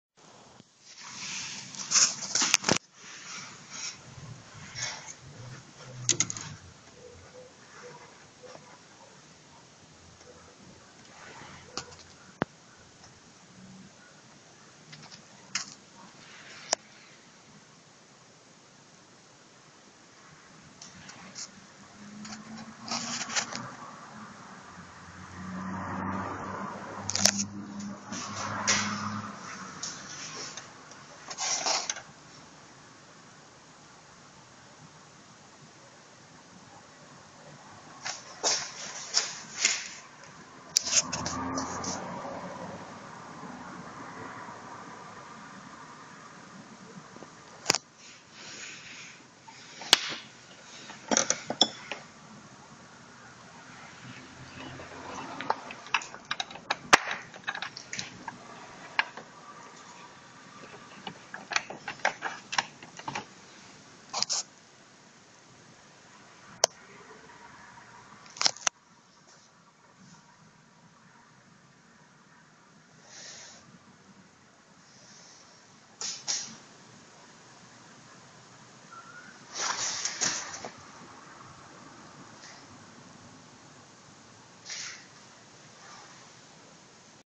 freeduino
После подключения через   usb  ethernet   шилд начитает работать...